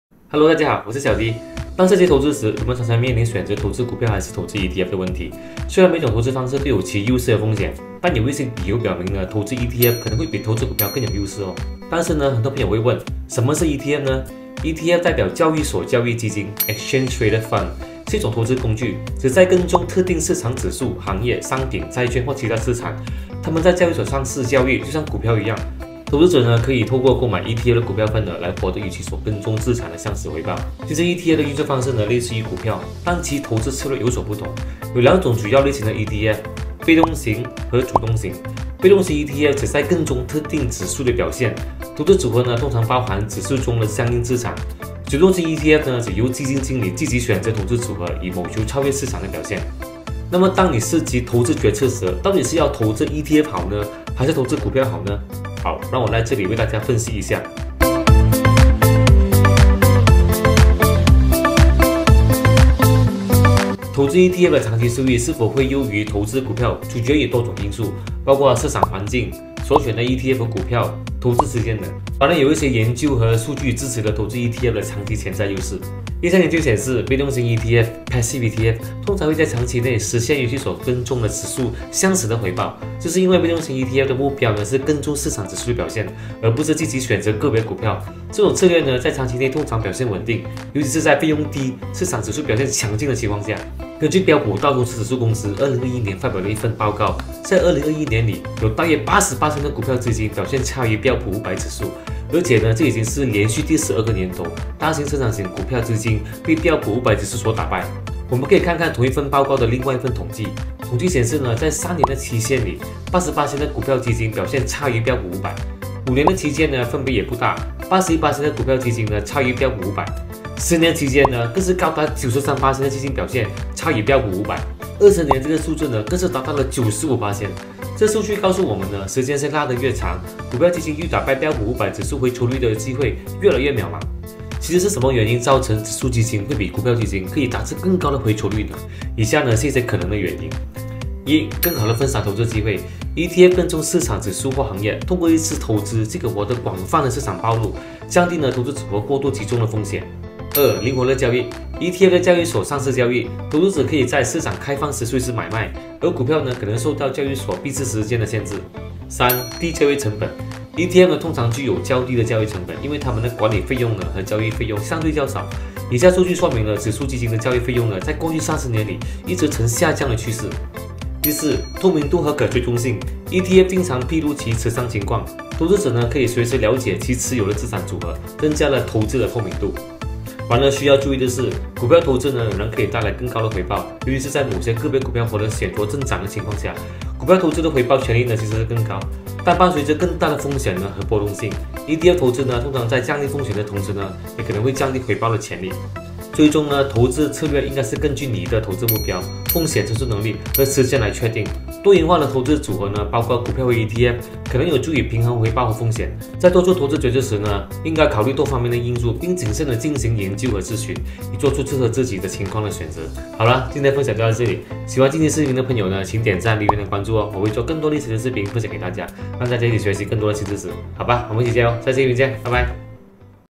别再投资股票了！｜ 投行大佬们不想你们知道的秘密｜完胜80%个股的神操作
当涉及投资时，人们常常面临选择投资股票还是投资ETF（交易所交易基金）的问题。虽然每种投资方式都有其优势和风险，但有一些理由表明投资ETF可能会比投资股票更有优势。

根据标普道琼斯指数公司2021年发表的一份报告。在2021年里，有大约80%的股票基金表现差于标普500指数。而且这已经是连续第12个年头，大型成长性股票基金被标普500指数打败。

其实是什么原因照成指数基金会比股票基金可以达致更高的回酬率更呢？以下是一些可能的原因。
🟢 更好的分散投资机会
🟢 灵活的交易
🟢 低交易成本
🟢 较低的管理费用